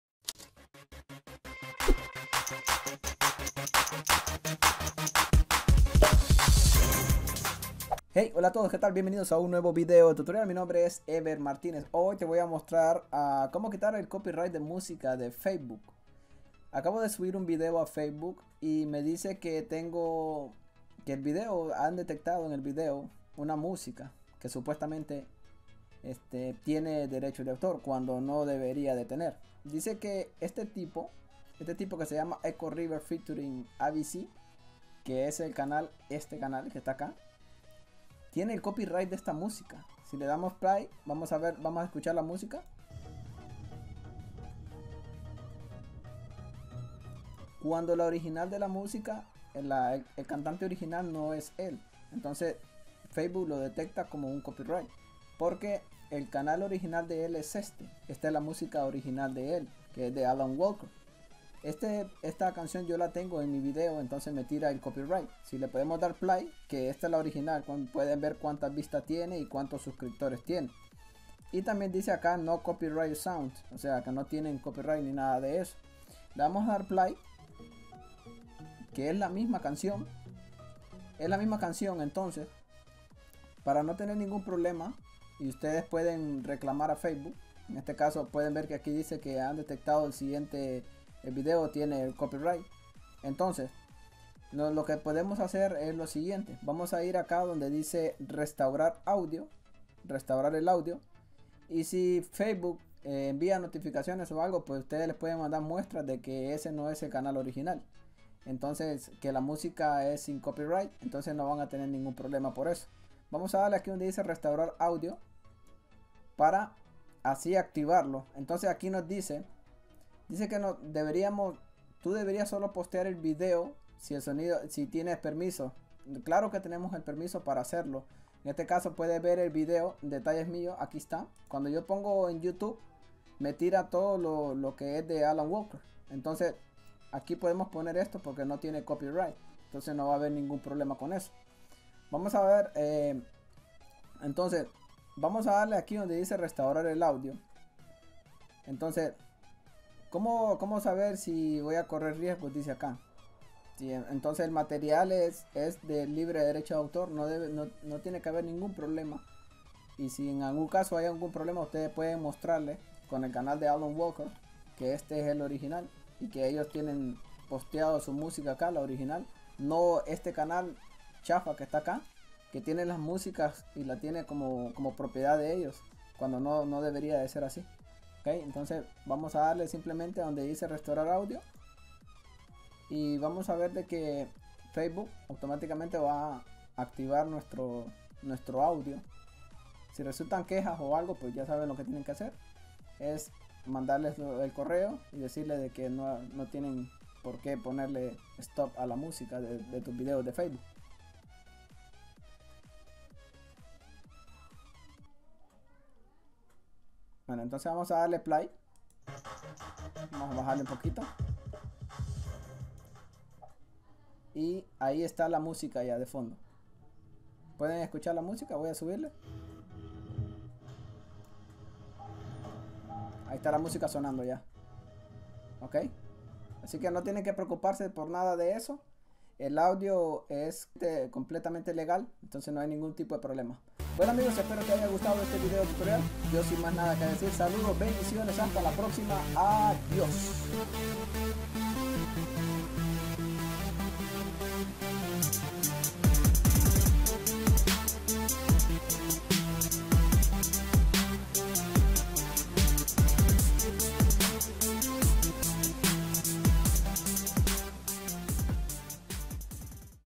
Alerta de Copy right de musica en facebook! Restablecer de nuevo!
◉  🏆[ -- ABRE AQUÍ -- ] 👍
En este video te muestro como volver a escuchar nuestra musica sin copy right :)

¡A Disfrutar!
Education
Gameplays
Tutoriales Android , PC ,Mac,iphone
Standard YouTube License
YD!

¡A Disfrutar!